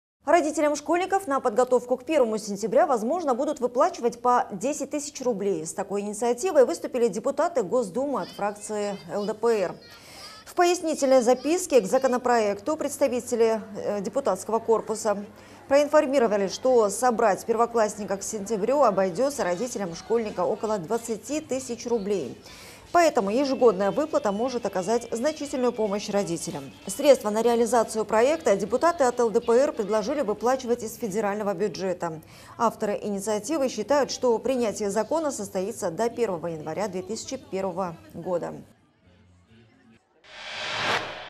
10 тыс. рублей к первому сентября?
Родителям школьников на подготовку к 1 сентября возможно будут выплачивать по 10 тыс рублей. С такой инициативой выступили депутаты Госдумы от фракции ЛДПР.

В пояснительной записке к законопроекту представители депкорпуса проинформировали, что собрать первоклассника к сентябрю обойдется родителям школьника около 20 тысяч рублей.
Поэтому ежегодная выплата может оказать значительную помощь родителям.

Средства на реализацию проекта депутаты от ЛДПР предложили выплачивать из федерального бюджета. Авторы инициативы считают, что принятие закона состоится до 1 января 2021 года.